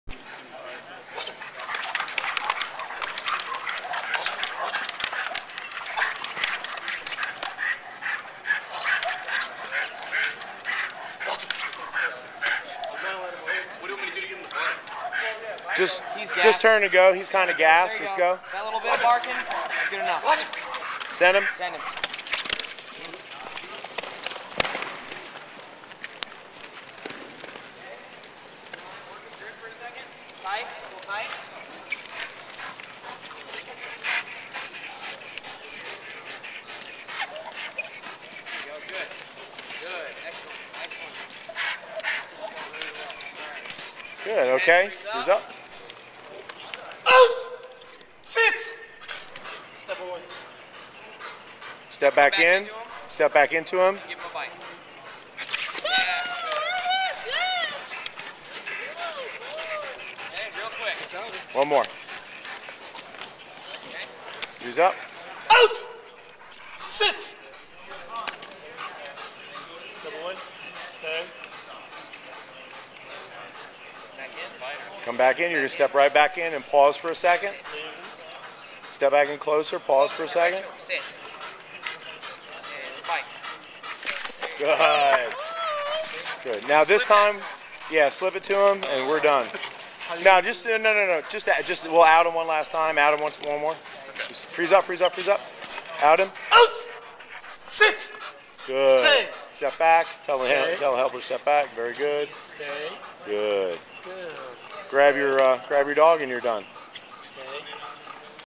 Out Progression 2: Double Line Field Out
In our decoy seminars we teach the students how to make a proper progression on the out. This video, using a student decoy, shows the second stage in the out progression using two lines to do field work.